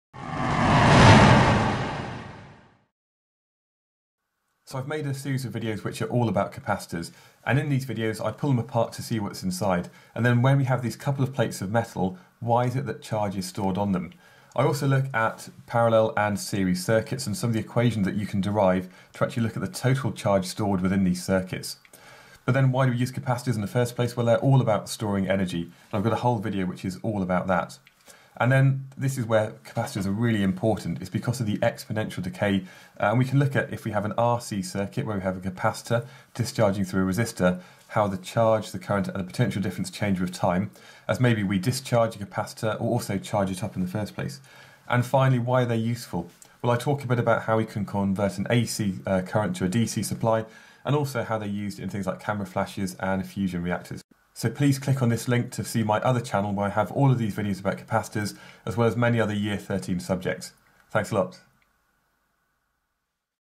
Capacitors - Everything you need to know! - A Level Physics
Capacitors
A capacitor is made from two metal plates separated by an insulator and their use in circuits is to store charge. The charge stored, Q, is directly proportional to the potential difference between the plates: Q = VC.

Capacitance is the charge stored per unit potential difference and is measure in farads where 1 farad is 1 coulomb per volt.

Capacitors in Series and Parallel
Capacitors store charge and can be connected in various combinations of series and parallel. This shows how you can derive the two capacitor equations (which are kind of the opposite to the equations you already know for resistors in series and parallel).

Energy Stored by a Capacitor
Capacitors store energy and the amount of energy, W, can be calculated if you know the capacitance of the capacitor, the potential difference across it or the charge stored on one of the plates. In this video you can see how the energy stored is equal to the area under a charge-potential difference graph.

Exponential Discharge of a Capacitor
Plotting a graph to show the discharge of a capacitor through a resistor leads onto an exponential decay graph - the same shape for the current, the charge stored and also the potential difference across it. This exponential decay graph has a constant-ratio of the decay per unit time which depends upon the time-constant for the circuit: the product of the capacitance of the capacitor and resistance of the circuit.

Capacitor Charging
What happens when you charge up a capacitor, how does the current vary with time and what about the charge stored and the potential difference across it? This video extends the content covered when you look at the discharge of a capacitor and the exponential decay characteristics of this component in a circuit.

Uses of Capacitors
Capacitors are not useful components to study because of exponential function of their charging and discharging. They are essential electrical components that can store charge in a circuit, with their advantage over cells and batteries in the fact they can be recharged and discharge many many times – unlike a rechargeable battery that has a limited lifespan.

Lewis